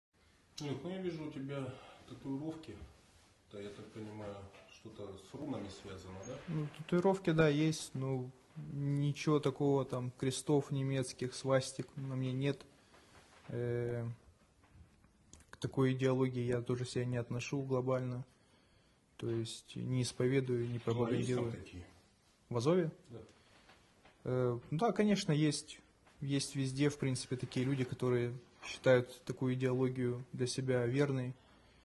Captured Azov soldier say he got the tattoos because they looked cool.
Russia-Ukraine War: Captured Azov guy in hospital. He says he got the tattoos because they looked cool but doesn't believe in the ideology.